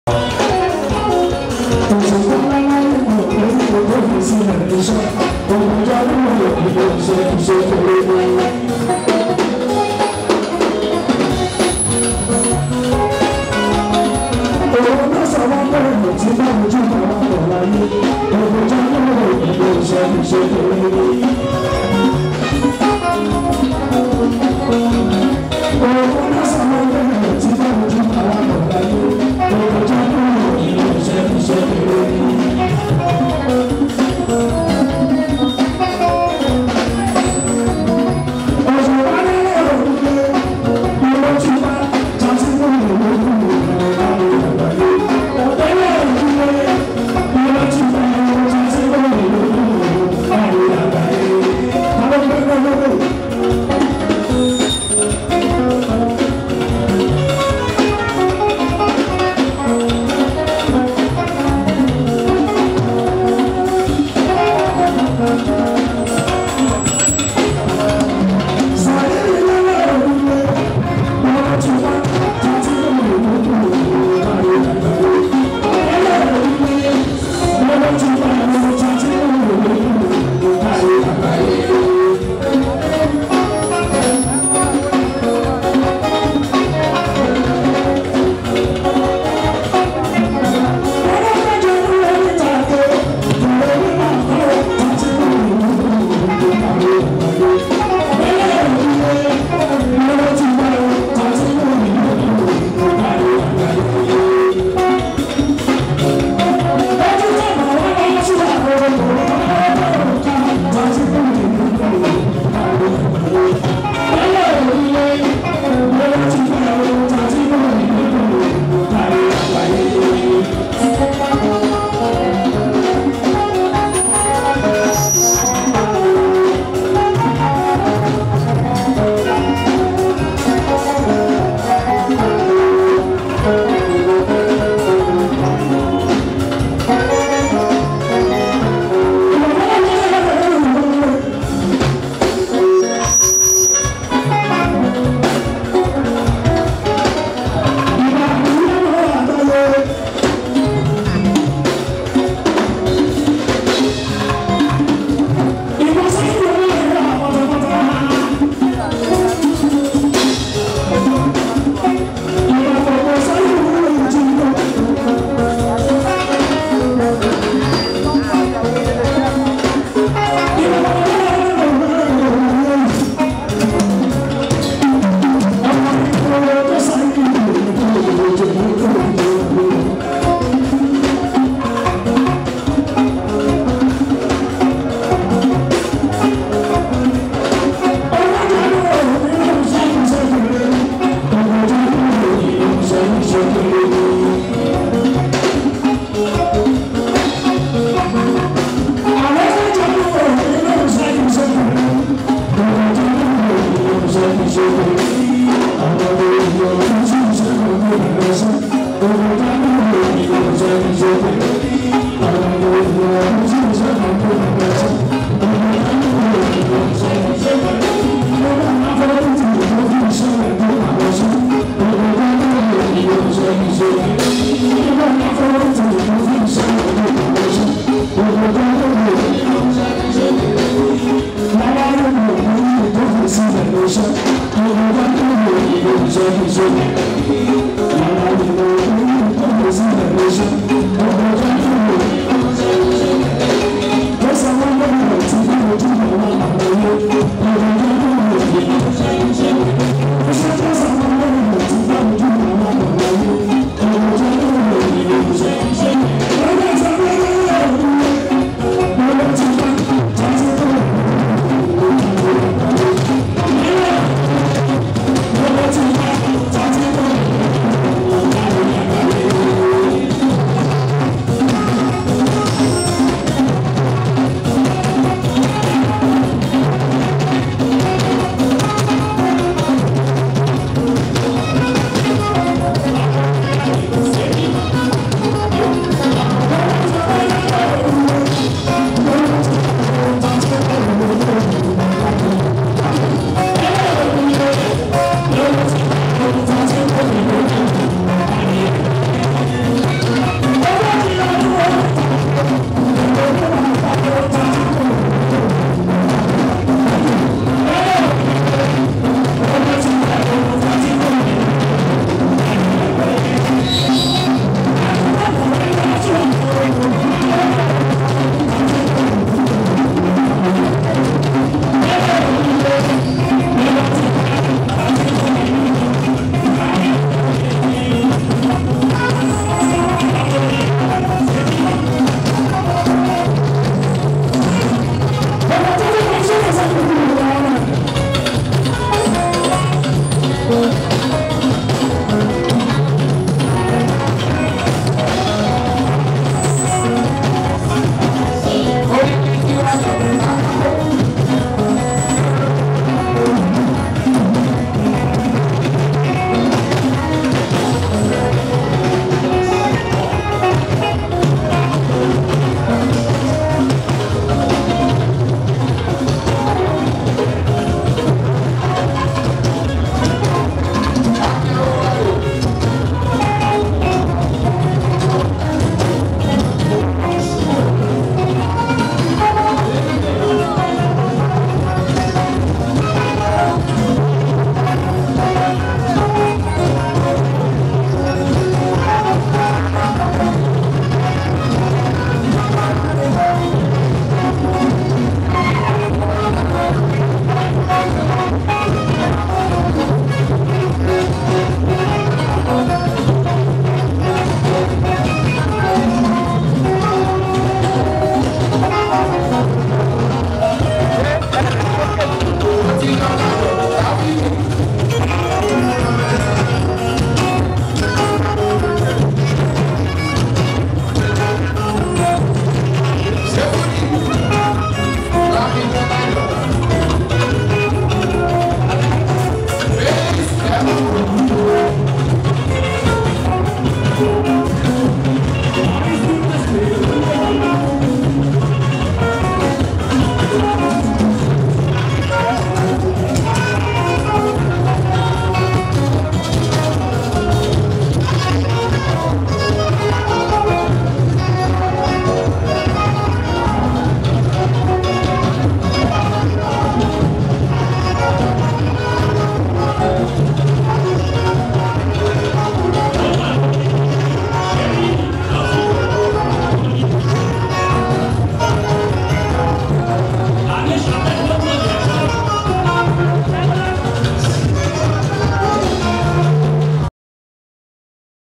King Saheed osupa and is humbleness live performance @ prince Dele Odule 40years on stage
Saheed osupa live performance